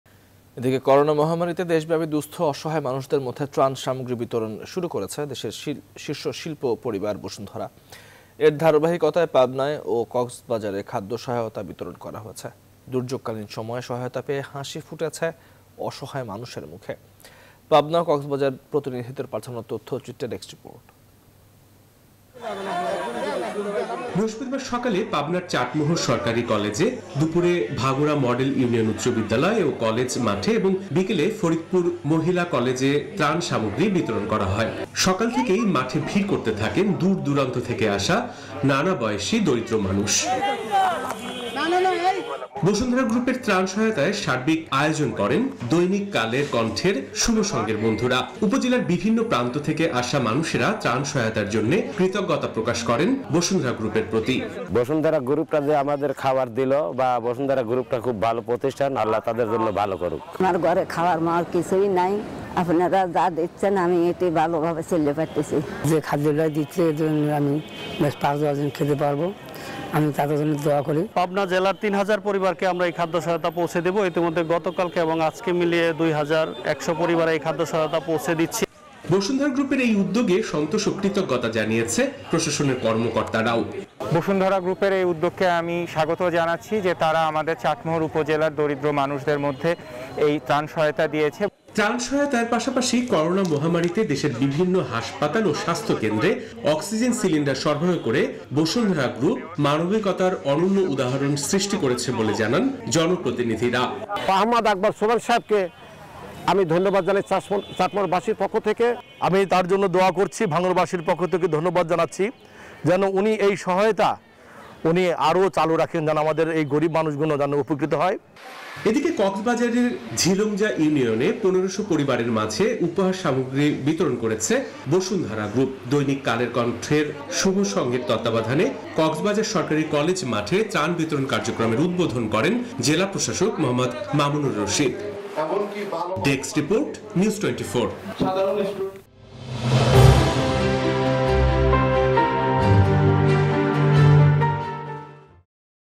দুর্যোগকালীন সময়ে বসুন্ধরার সহায়তা পেয়ে হাঁসি ফুটেছে পাবনা ও কক্সবাজারের অসহায় মানুষের মুখে13Aug.21
করোনা মহামারীতে দেশব্যাপী দু:স্থ অসহায় মানুষদের মধ্যে ত্রান সামগ্রী বিতরন শুরু করেছে দেশের শীর্ষ শিল্প পরিবার বসুন্ধরা। এর ধারাবাহিকতায় পাবনায় ও কক্সবাজারে খাদ্য সহায়তা বিতরণ করা হয়েছে। দুর্যোগকালীন সময়ে সহায়তা পেয়ে হাঁসি ফুটেছে অসহায় মানুষের মুখে। পাবনা ও কক্সবাজার প্রতিনিধির পাঠানো তথ্যচিত্রে ডেস্ক রিপোর্ট।
বৃহস্পতিবার সকালে চাটমোহর সরকারি কলেজে দুপুরে ভাঙ্গুড়া মডেল ইউনিয়ন উচ্চ বিদ্যালয় ও কলেজ মাঠে এবংবিকেলে ফরিদপুর মিহলা কলেজে ত্রান সামগ্রি বিতরন করা হয়।  সকাল থেকেই মাঠে ভীড় করেন দুর দূরান্ত থেকে আসা নানা বয়সী দরিদ্র মানুষ। এরপর স্বাস্থবিধি মেনে তারা অবস্থান নেন ত্রাণ সহায়তার প্যাকেটের পাশে। একাজে সহায়তা করেন দৈনিক কালের কণ্ঠের শুভসংঘের বন্ধুরা। উপজেলার বিভিন্ন প্রান্ত থেকে আসা মানুষেরা ত্রাণ সহায়তার জন্য কৃতজ্ঞতা প্রকাশ করেছেন বসুন্ধরা গ্রুপের প্রতি।
ভক্সপপ ১,২,৩,৪,৫,৬,৭,৮
বসুন্ধরা গ্রুপের এই উদ্যোগে সন্তোষ ও কৃতজ্ঞতা জানিয়েছেন প্রশাসনের কর্মকর্তা ও জনপ্রতিনিধিরা।
সিংক ১ আব্দুল হামিদ মাষ্টার, উপজেলা চেয়ারম্যান
সিংক ২ সৈকত ইসলাম,  উপজেলা নির্বাহী কর্মকর্তা, চাটমোহর
ত্রান সহায়তার পাশাপাশি করোনা মহামারীতে দেশের বিভিন্ন হাসপাতাল ও স্বাস্থ্য কেন্দ্রে অক্সিজেন সিলিন্ডার সরবরাহ করে বসুন্ধরা গ্রুপ মানবিকতার অনন্য উদাহরন সৃষ্টি করেছে বলে জানান জনপ্রতিনিধিরা।
সিংক ৩, গোলাম হাসনায়েন রাসেল, মেয়র ভাঙ্গুড়া পৌরসভা।

এদিকে কক্সবাজারের ঝিলংজা ইউনিয়ন ১৫০০ পরিবারের মাঝে উপহার সামগ্রী বিতরণ করেছে বসুন্ধরা গ্রুপ। দৈনিক কালের কণ্ঠের শুভসংঘের তত্বাবধানে কক্সবাজার সরকারি কলেজ মাঠে ত্রাণ বিতরণ কার্যক্রমের উদ্বোধন করেন জেলাপ্রশাসক মো. মামুনুর রশীদ। কালের কণ্ঠের বিশেষ প্রতিনিধি তোফায়েল আহমদের সভাপতিত্বে অনুষ্ঠানে বিশেষ অতিথি ছিলেন  আওয়ামী লীগের ধর্মবিষয়ক সম্পাদক অ্যাডভোকেট সিরাজুল মোস্তফা, কক্সবাজার সরকারি কলেজের অধ্যক্ষ কামরুল আহসান, অতিরিক্ত জেলা প্রশাসক জাহিদ ইকবাল ও সদর উপজেলার নির্বাহী কর্মকর্তা মিল্টনরায়।

Our other YouTube channels :
News24 Official Address:
NEWS24
371/A
Bashundhara Road
Block – D
Bashundhara Residential Area
Dhaka - 1229